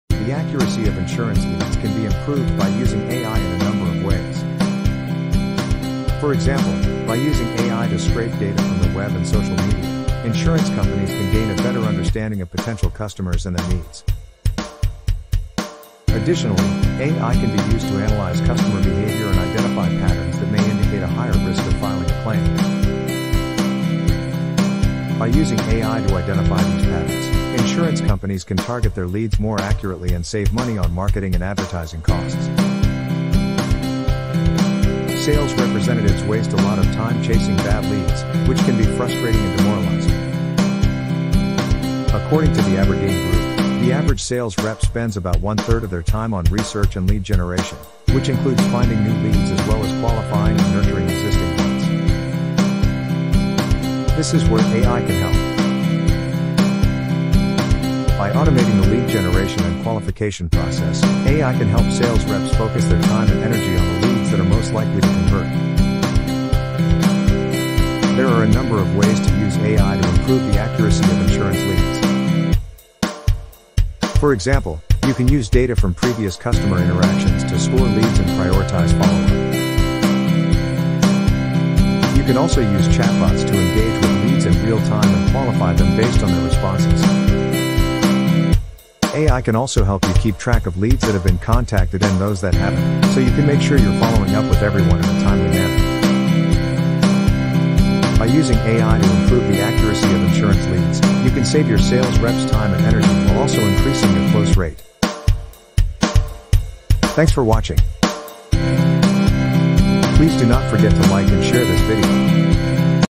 HOW AI CAN IMPROVE THE ACCURACY OF INSURANCE LEADS
The accuracy of insurance leads can be improved by using AI in a number of ways. For example, by using AI to scrape data from the web and social media, insurance companies can gain a better understanding of potential customers and their needs. Additionally, AI can be used to analyze customer behavior and identify patterns that may indicate a higher risk of filing a claim. By using AI to identify these patterns, insurance companies can target their leads more accurately and save money on marketing and advertising costs. Sales representatives waste a lot of time chasing bad leads, which can be frustrating and demoralizing. AI chatbots can be used to engage leads, and AI can also be used to track lead.
insurnace agents; # insurance companies, # insurance brokers